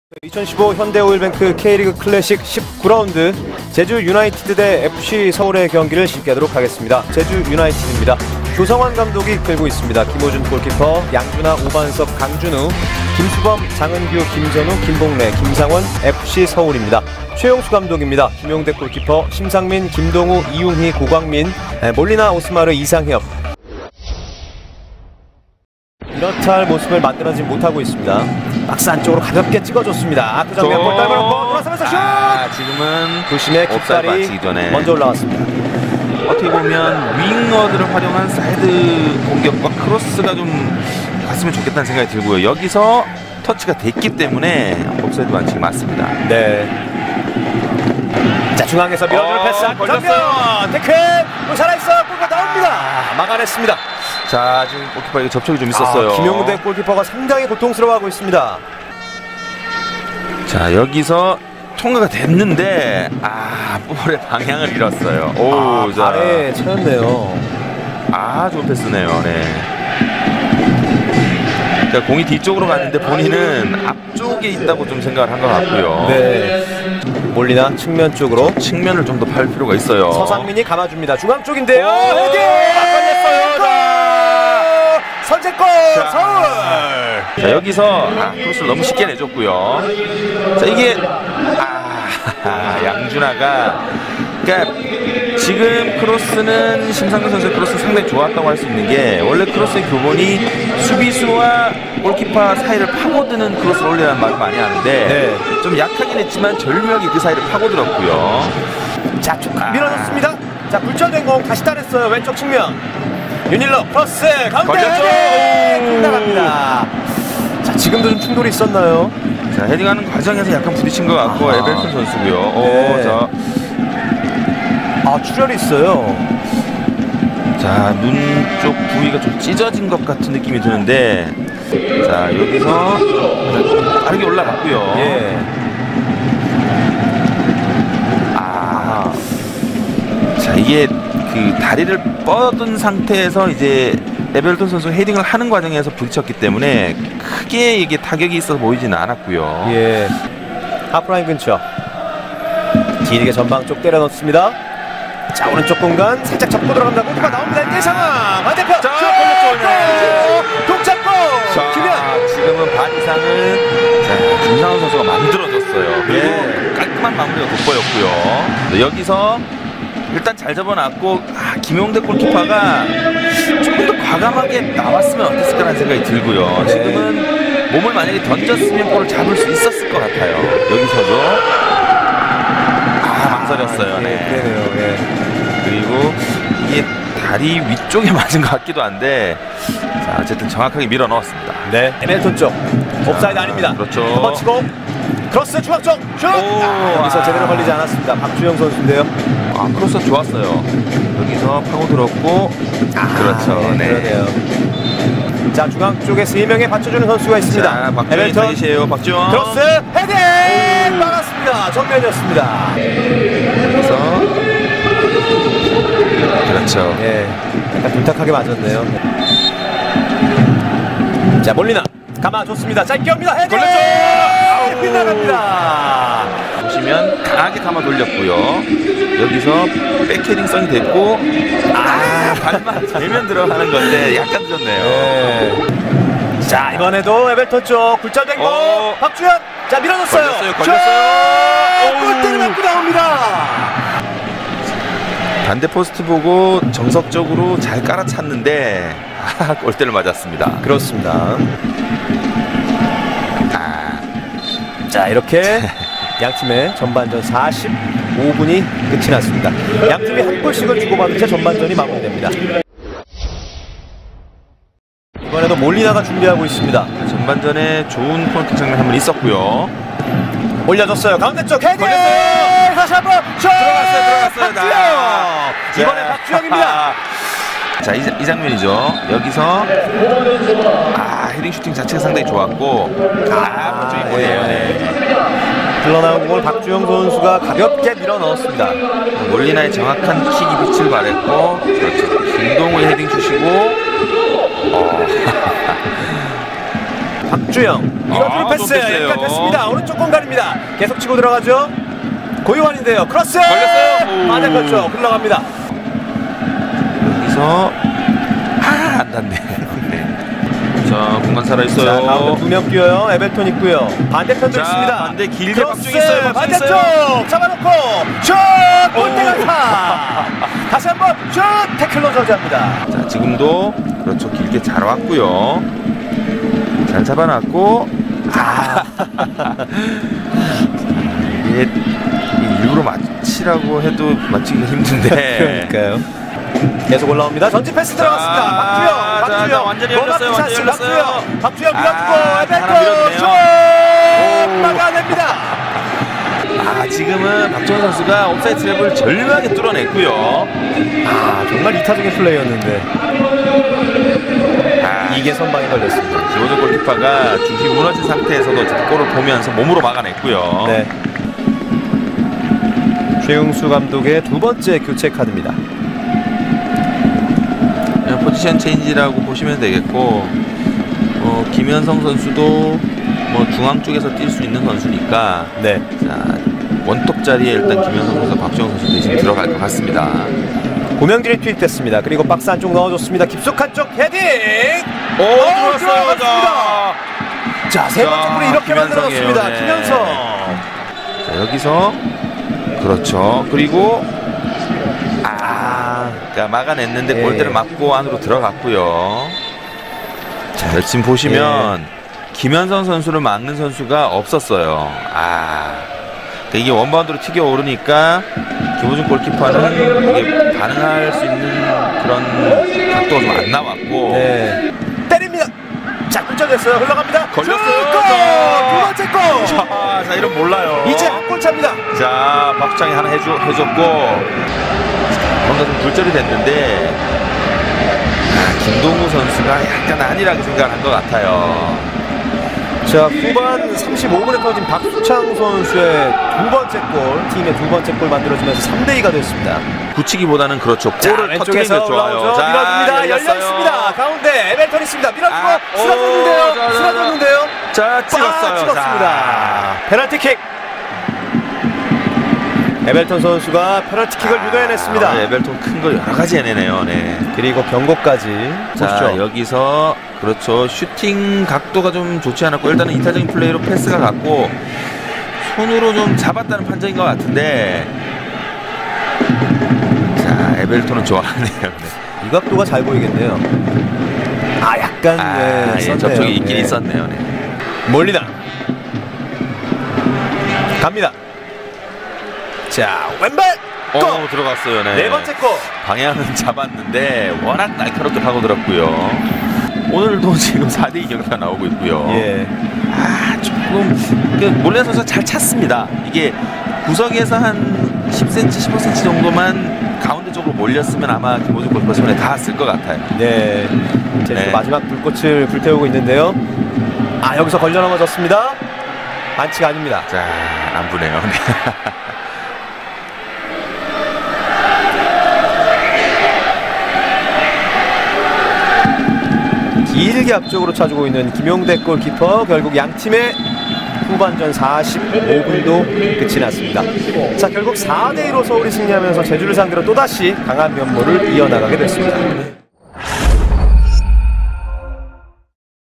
하이라이트 - FC서울 vs 제주 (A)
2015년 7월 1일에 있었던 K LEAGUE​ 클래식 19R 제주유나이티드​와의 원정경기 하이라이트입니다.

이날 경기는 4-2로 승리하며 제주전 무패 행진을 이어갔습니다.